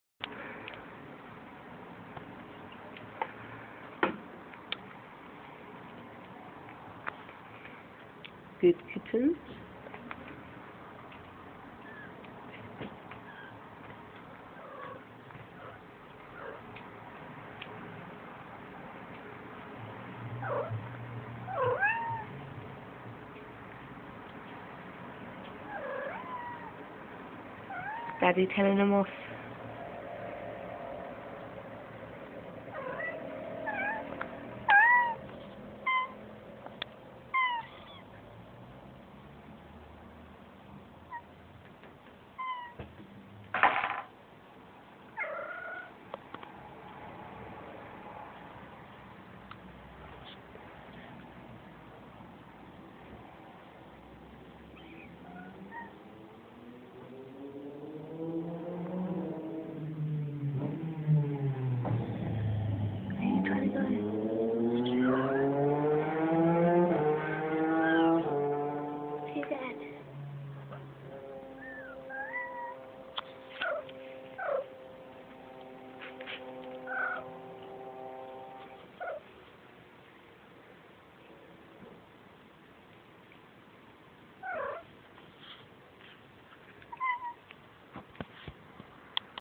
daddy plays his bit in family
heres sirocco showing us how good a dad he is